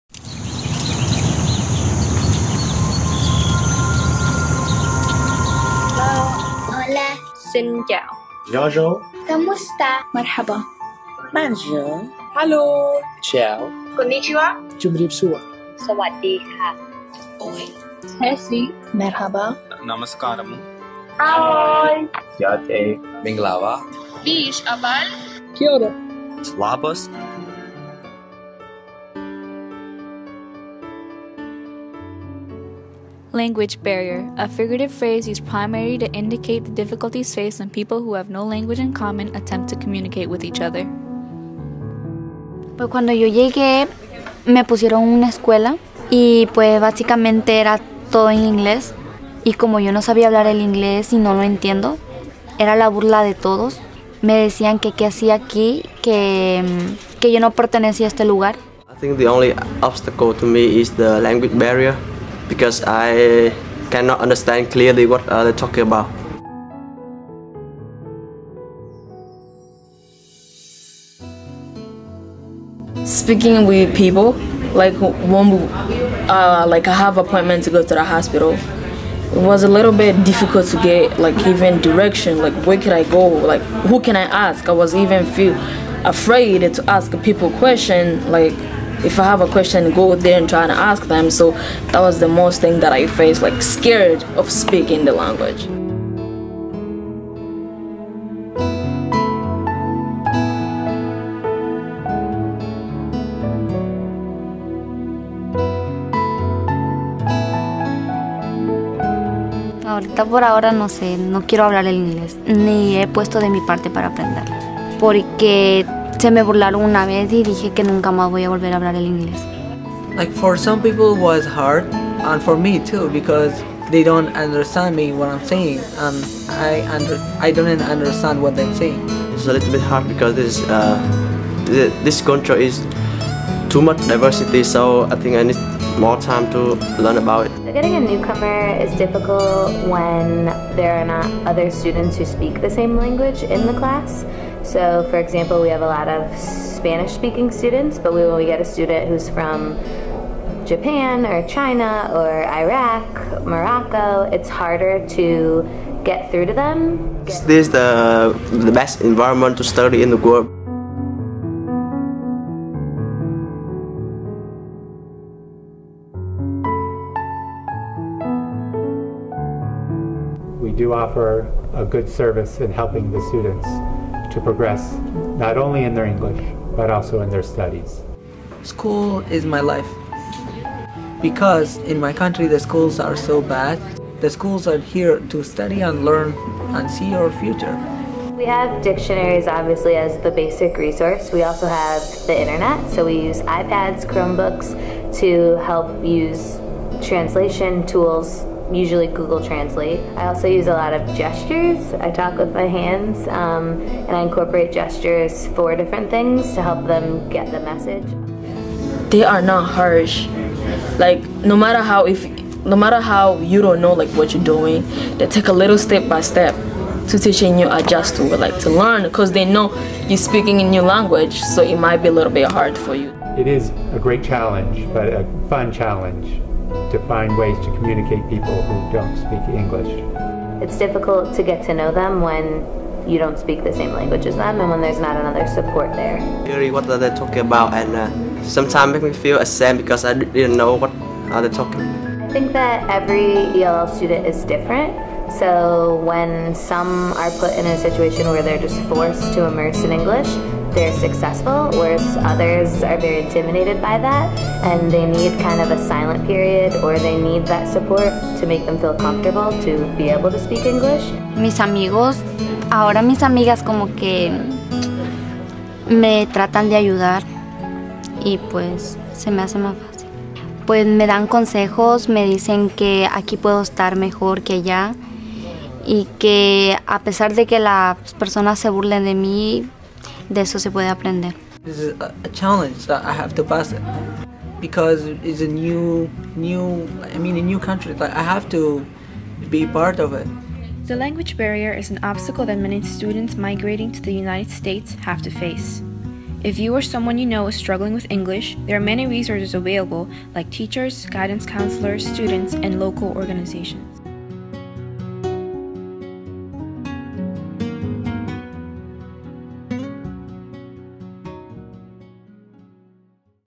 Barriers Documentary
A short documentary about the language barrier students face as they enter U.S. schools speaking little or no English.
The film was produced by students of the International Academy of Digital Arts and Sciences at Wilbur Cross High School in New Haven, CT for entry in the Connecticut Student Film Festival.